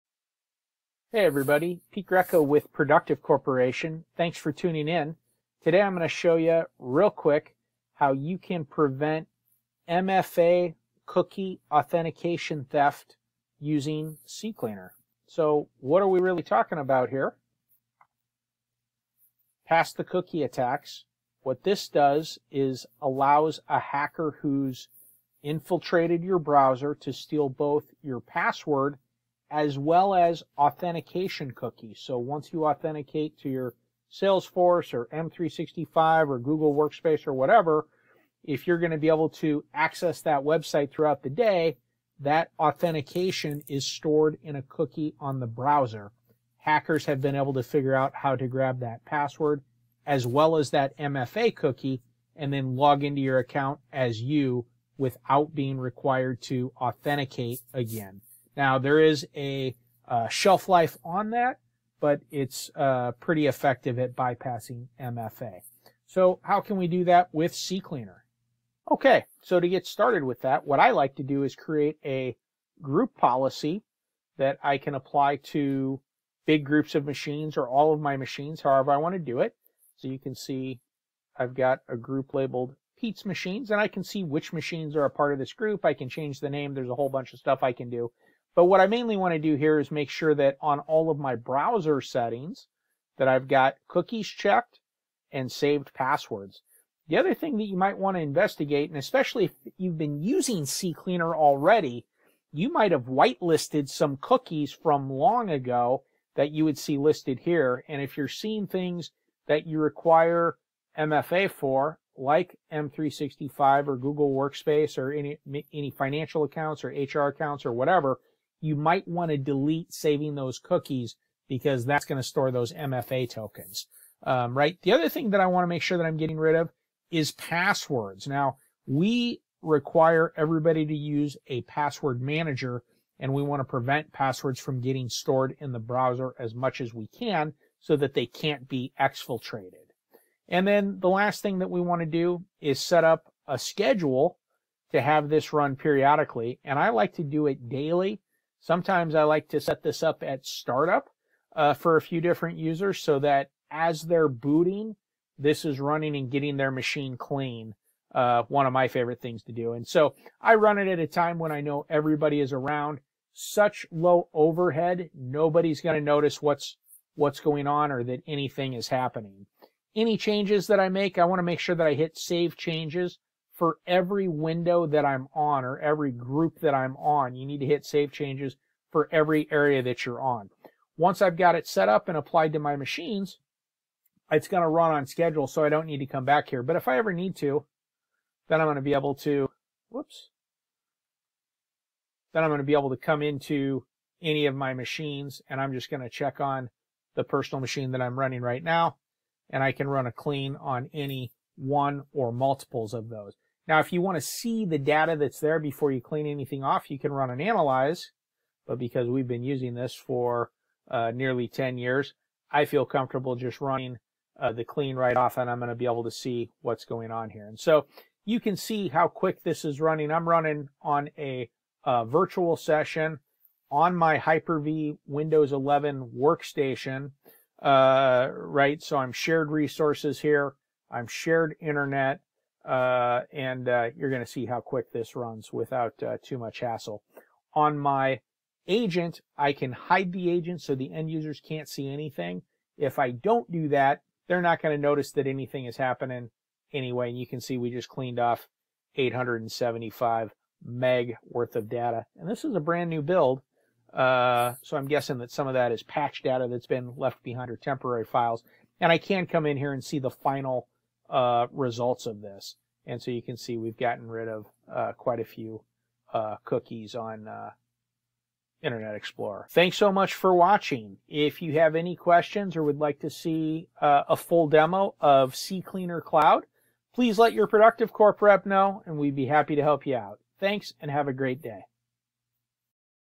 Clearing Cookies and Passwords with CCleaner
Come see why it is important to clean your cookies and passwords from your cache